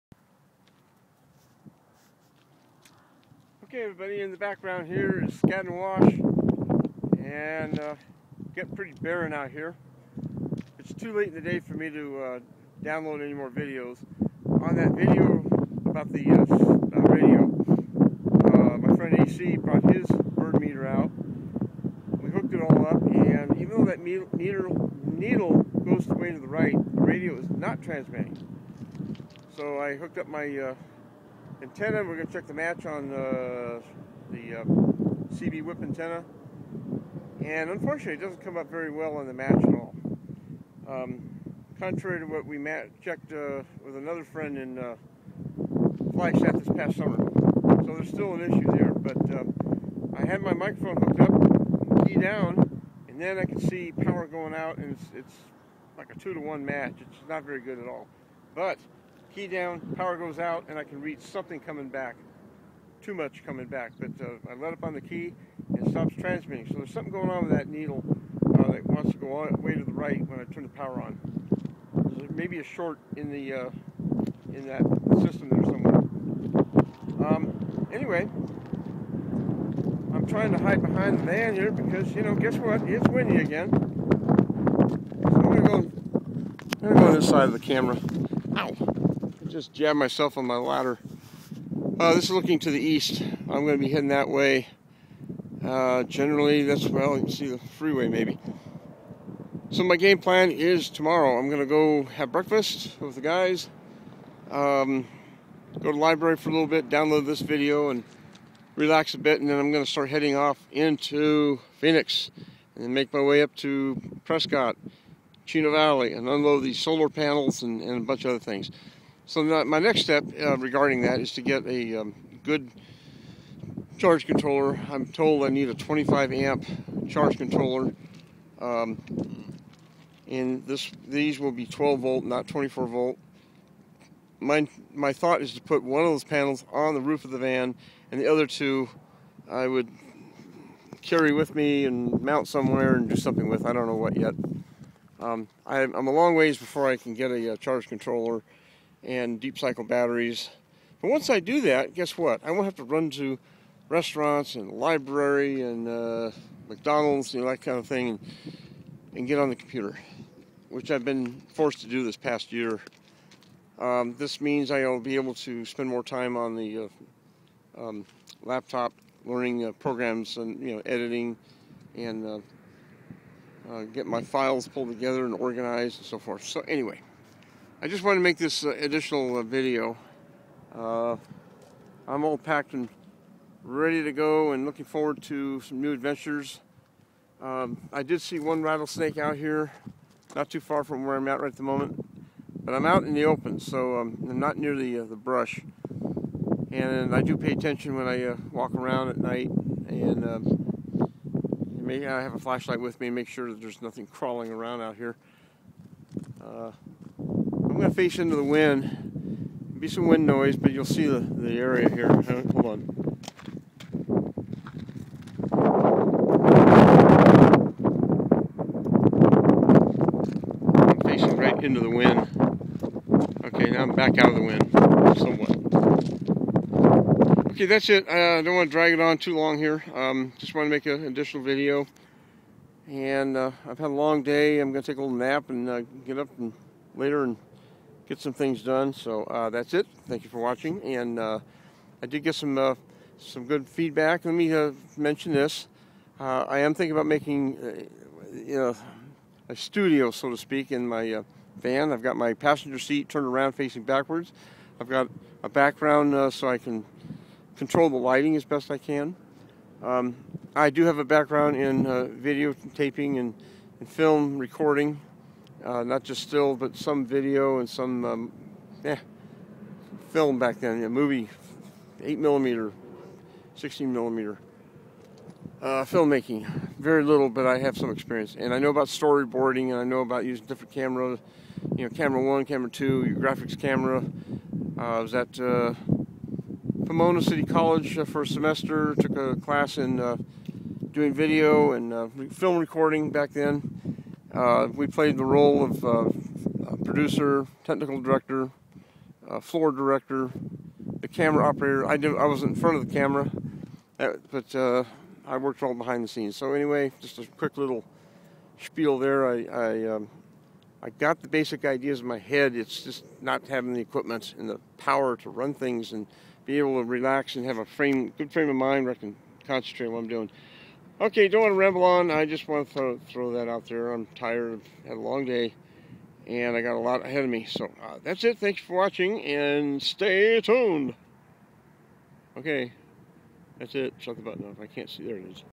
The Nomadic Ham
More thoughts to share with you all.  Saw one rattle snake in the area. They are coming out, but I am leaving soon.